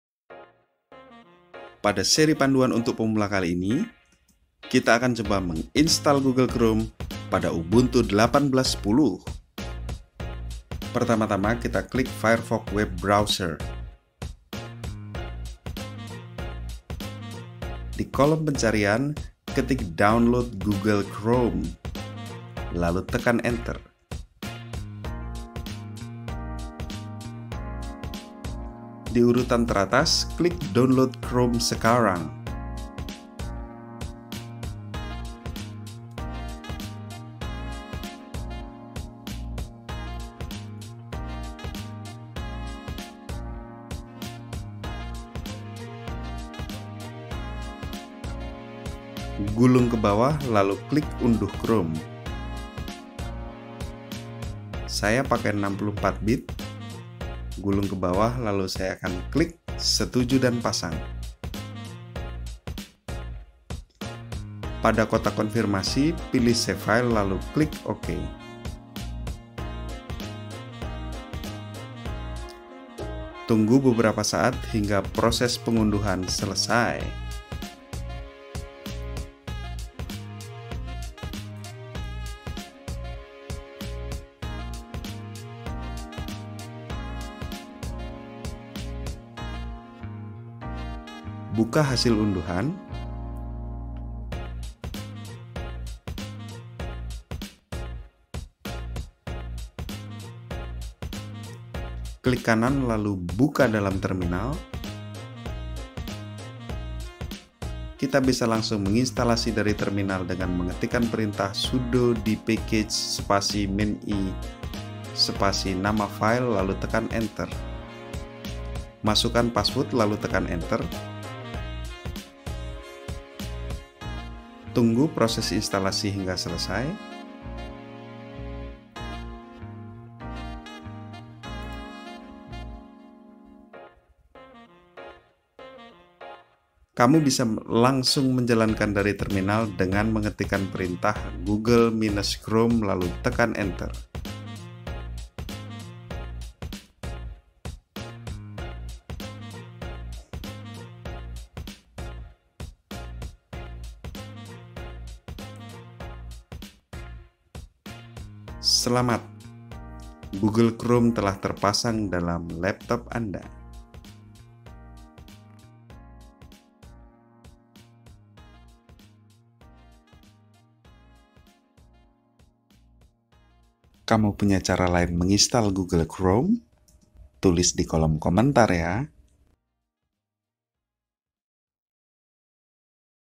Ubuntu tutorial for beginners 2019 - Install Google Chrome pada Ubuntu 18 10
Menurut Stat Counter, Google Chrome adalah Web browser yang paling banyak digunakan di dunia.  Salah satu fitur yang saya sukai adalah kemudahan sinkronisasi dengan android.
Berikut ini cara Install Google Chrome di Ubuntu 18.10

Alat perekaman:
Video editor: Openshot
Mic : Asus Zenfone Selfie Lite
Versi Ubuntu: 18.10 (Cosmic Cuttlefish)